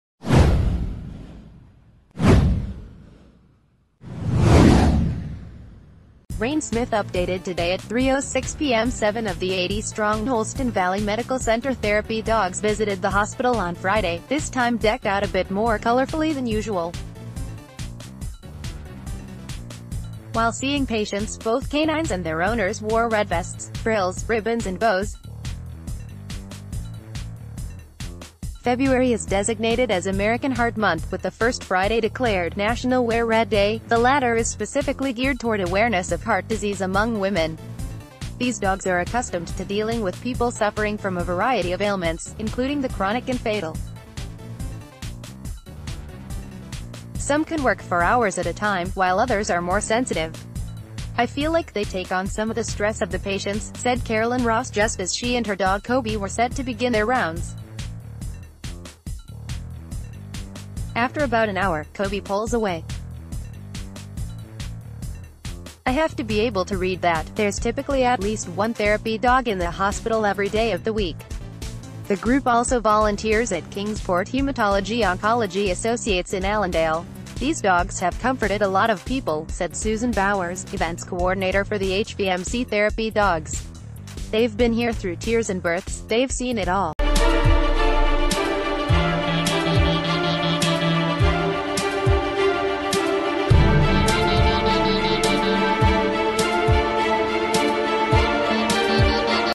Therapy dogs don red for heart awareness at HVMC
Rain Smith • Updated Today at 3:06 PM Seven of the 80-strong Holston Valley Medical Center Therapy Dogs visited the hospital on Friday -- this time decked out a bit more colorfully than usual. While seeing patients, both canines and their owners wore red vests, frills, ribbons and bows. February is designated as American Heart Month, with the first Friday declared "National Wear Red Day." The latter is specifically geared toward awareness of heart disease among women. These dogs are accustomed t...